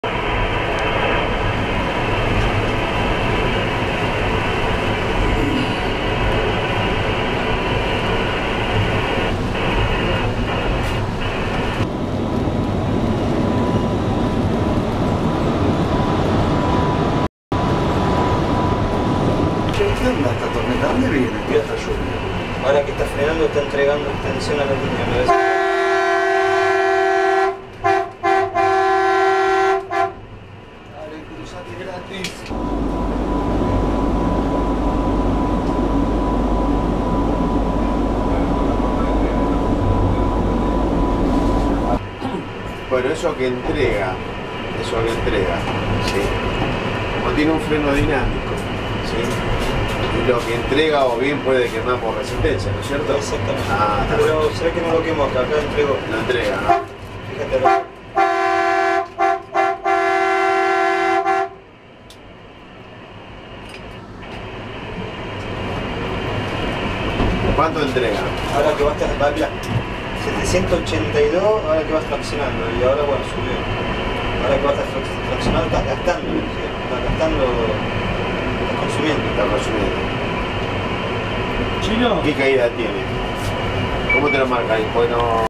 Prueba dinámica Nocturna del Tren Sarmiento
Primer prueba dinámica nocturna del tren Sarmiento bajo la lluvia, a 60 km horarios.